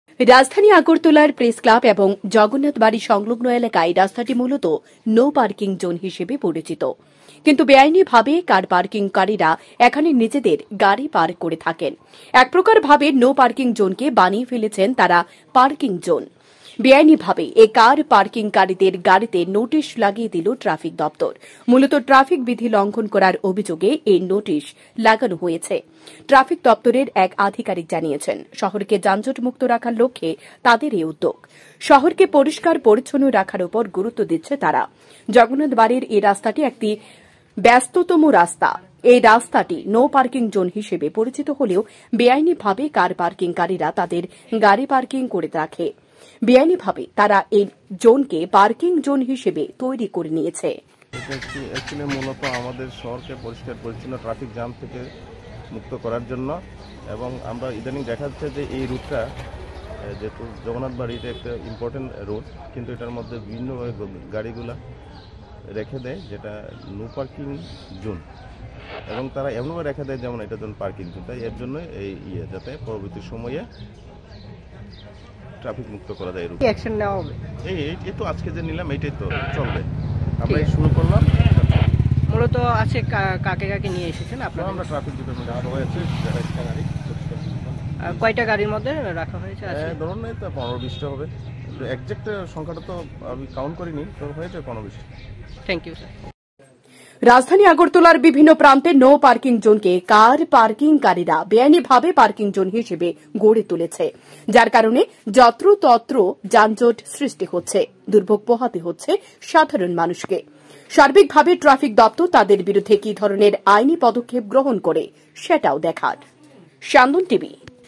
আগরতলা প্রেসক্লাব এবং জগন্নাথ বাড়ির সামনে নৌ পার্কিং জোনে কার পার্কিং কারীদের বিরুদ্ধে
আগরতলা প্রেসক্লাব এবং জগন্নাথ বাড়ির সামনে নৌ পার্কিং জোনে কার পার্কিং কারীদের বিরুদ্ধে সরব হল ট্রাফিক দপ্তর